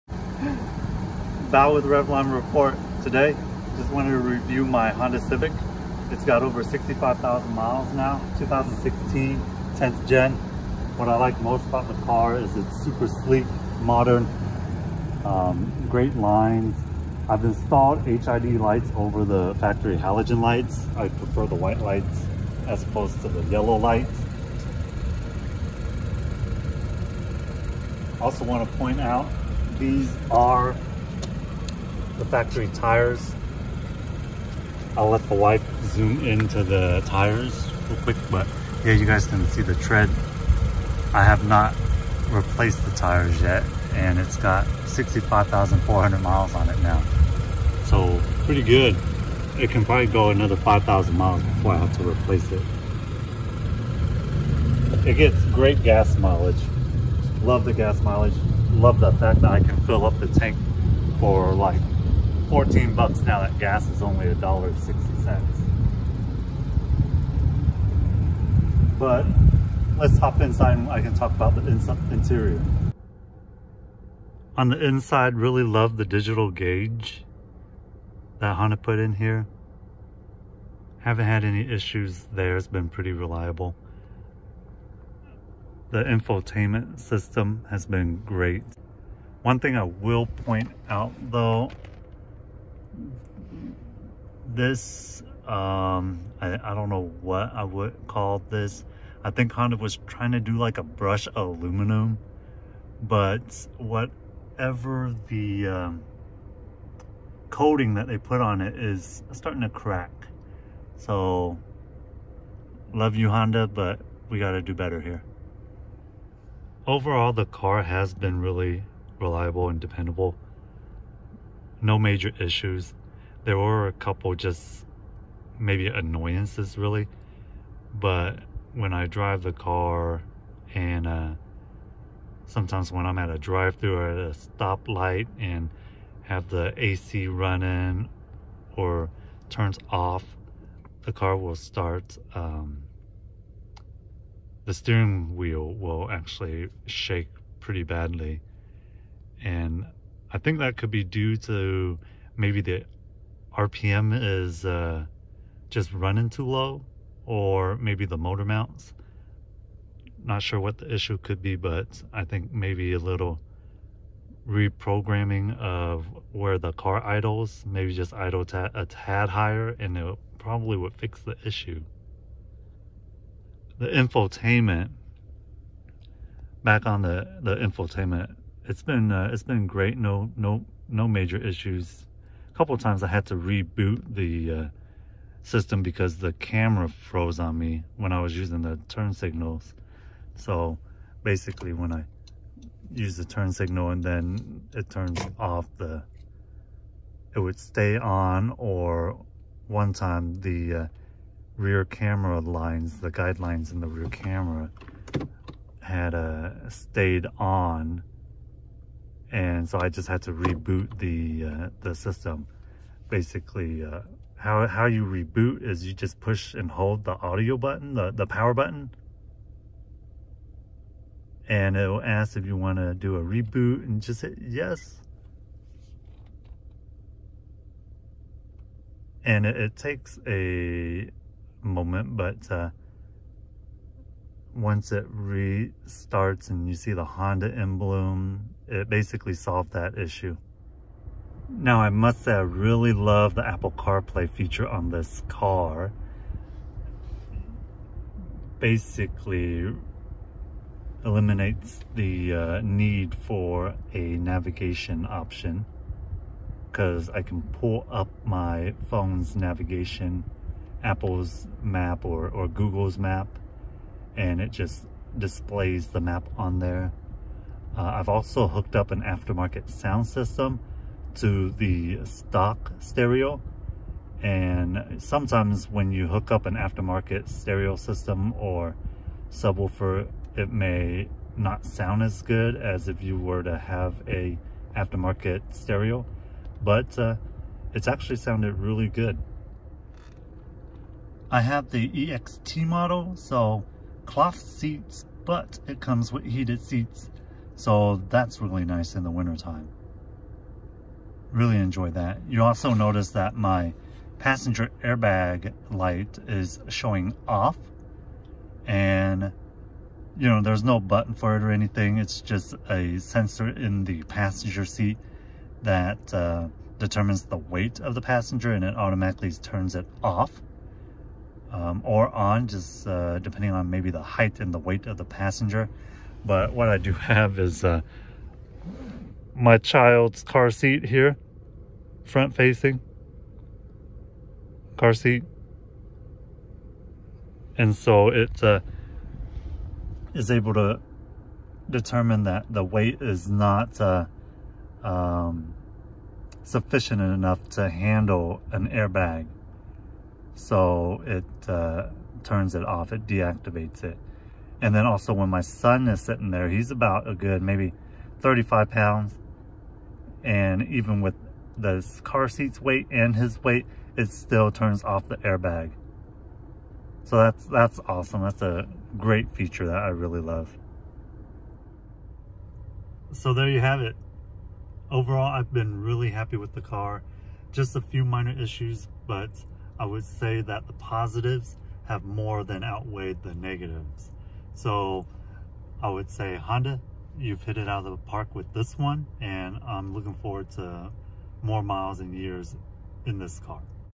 10th Gen 2016 Honda Civic Review | How to Reboot Infotainment System
Reviewing my 10th Gen 2016 Honda Civic. I talk about some negatives and some of the things I love about this car. I also discuss a couple infotainment issues I had and how to reboot the system.